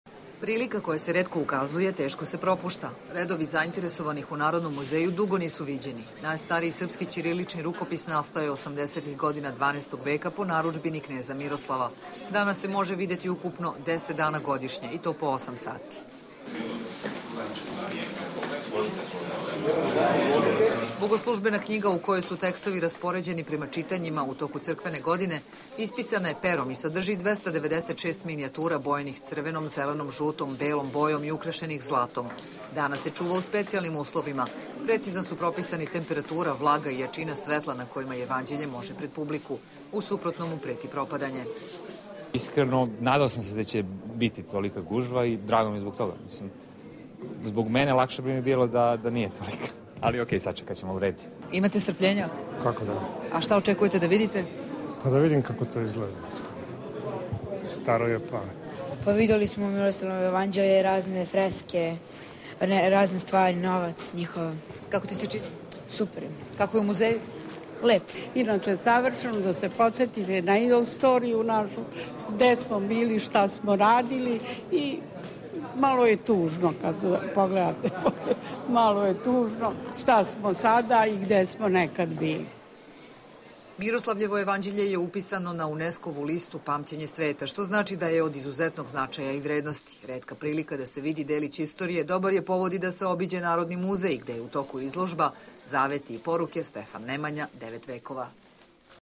Redovi za Miroslavljevo jevandelje
Najstariji i najvredniji sačuvani srpski ćirilični rukopis Miroslavljevo jevandelje - po prvi put ove godine predstavljen je javnosti.